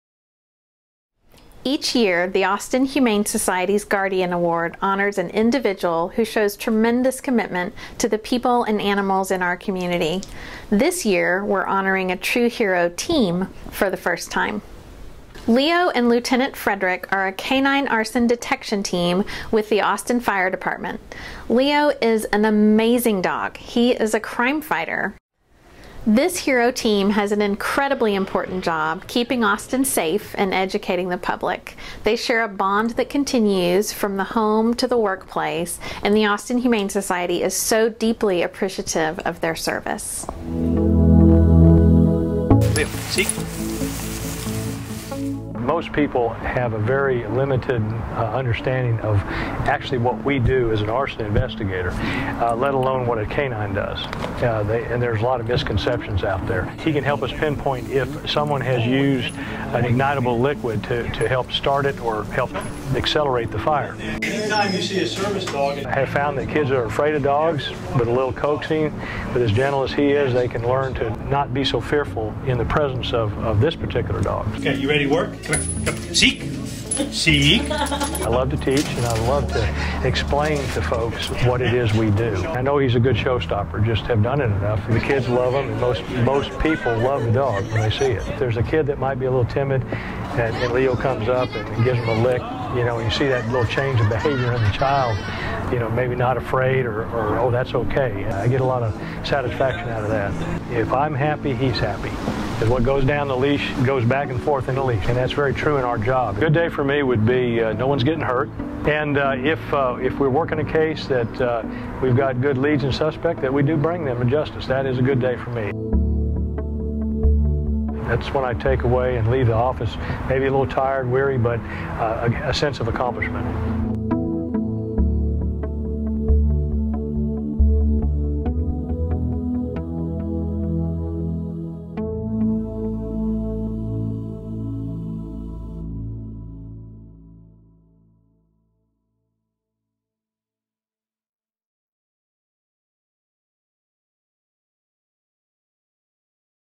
Guardian Award 2012 - Austin Humane Society
The Austin Humane Society 2012 Guardian Award winners are Leo & Lt. Brooks Frederick. The two serve as a canine arson detection team with the Austin Fire Department. Leo is crime fighter, who helps solve mysteries. He is trained to sniff out and pinpoint whether someone has used an ignitable liquid to start or accelerate an arson fire. Leo also serves as an ambassador for public safety education and accompanies Lt. Frederick on school visits. The AHS selected the team for their exemplary dedication to their jobs, for helping keep Austin safe and their commitment to educating the public about fire safety. Leo and Lt. Frederick share a bond that continues from their home to the workplace, and our community is deeply appreciative of their service.

Shot/Written/Produced/Edited by Kelly Hanes